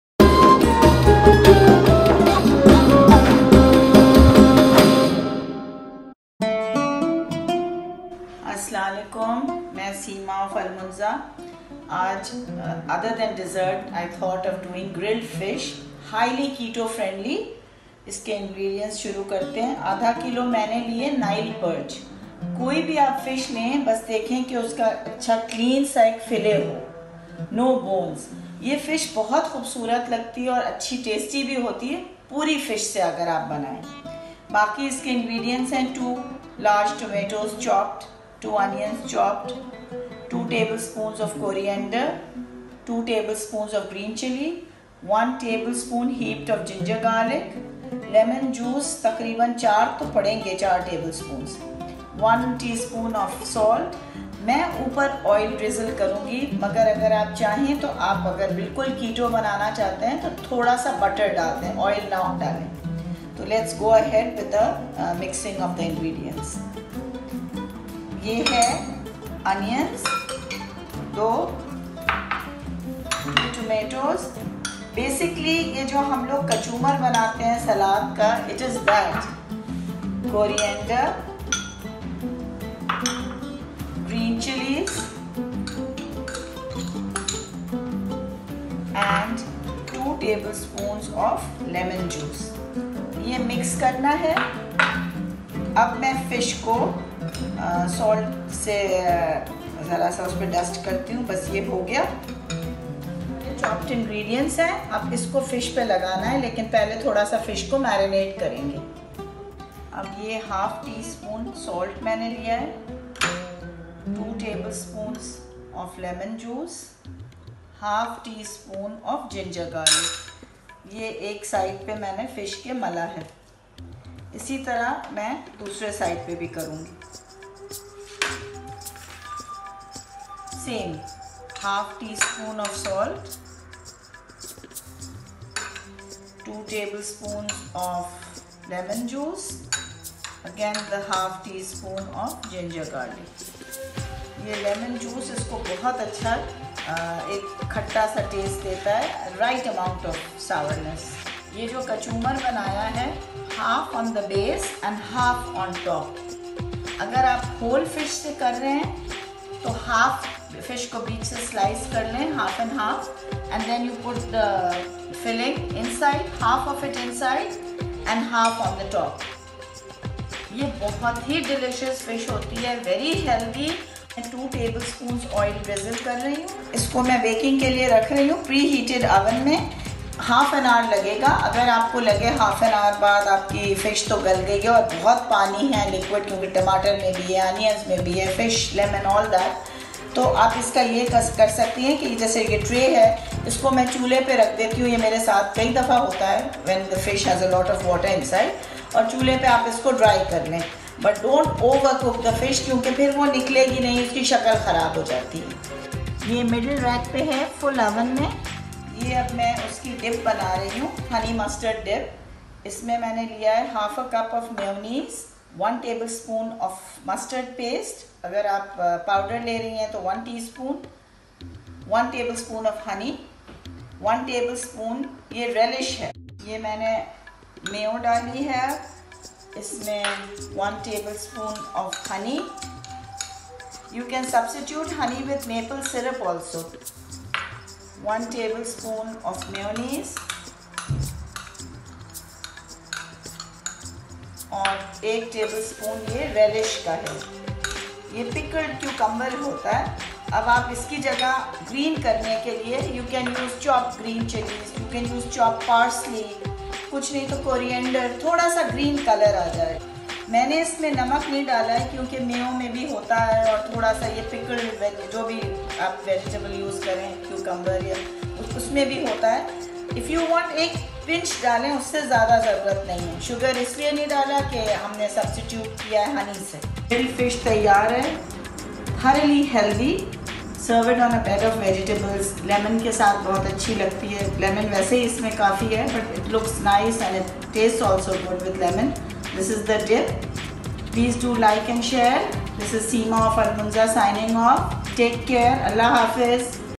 KETO Salsa Fish!! Healthy and delicious!! (In Urdu / Hindi)
Fish & Salsa
Half kg any fish boneless fillet
2 Onions chopped
2 Tomatoes  chopped
2 Tbsp chopped  coriander
2 Tbs chopped green chillies
1 Tsp salt
4 Tbs lemon juice
1 Tbs ginger  n garlic
2 Tbs oil

Honey mustard  dip:
Half cup mayonnaise
1 tbs mustard
1 tbs honey
1 tbs cucumber  relish salty if required